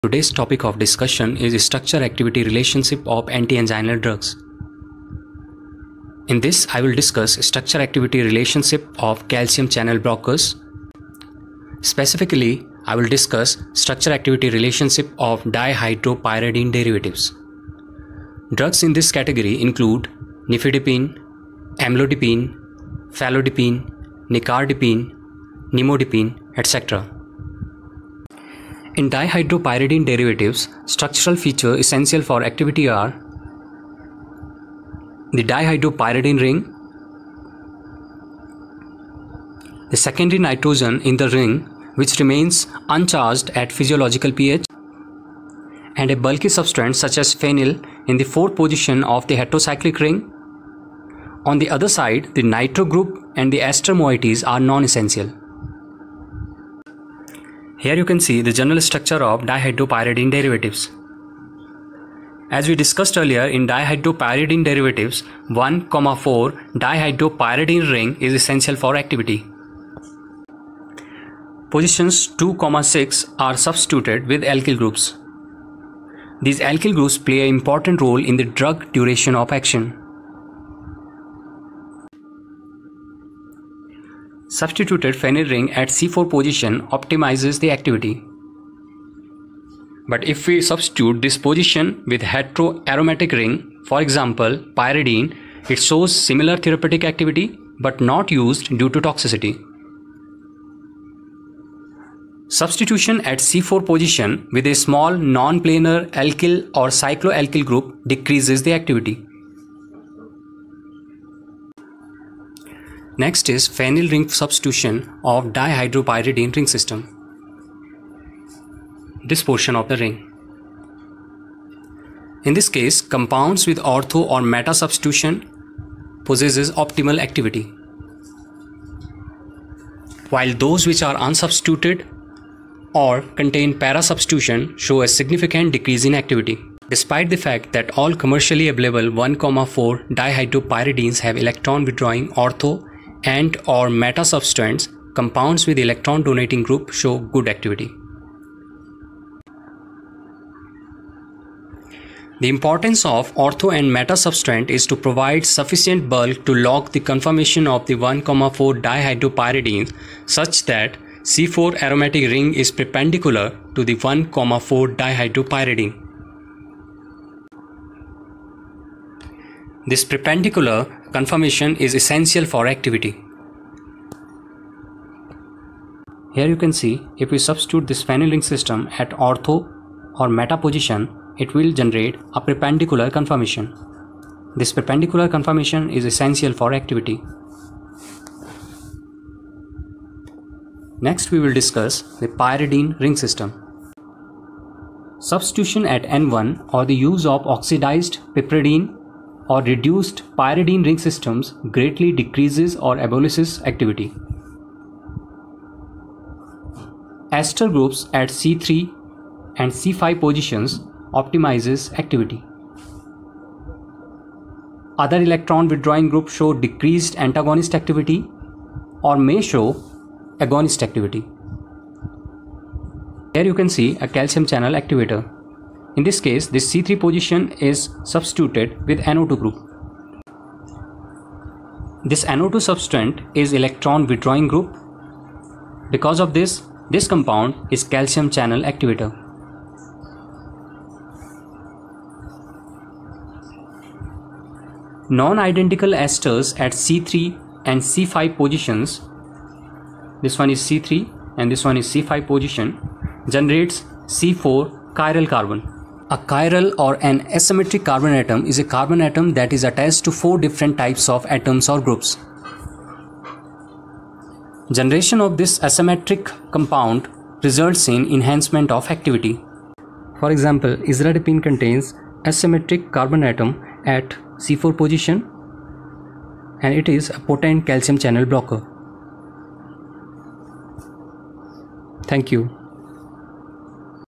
Antianginal Drugs: SAR of Calcium channel blockers
Antianginal Drugs: SAR of Calcium channel Blockers,@PharmTechInsights

This video presentation lecture contains SAR of calcium channel blockers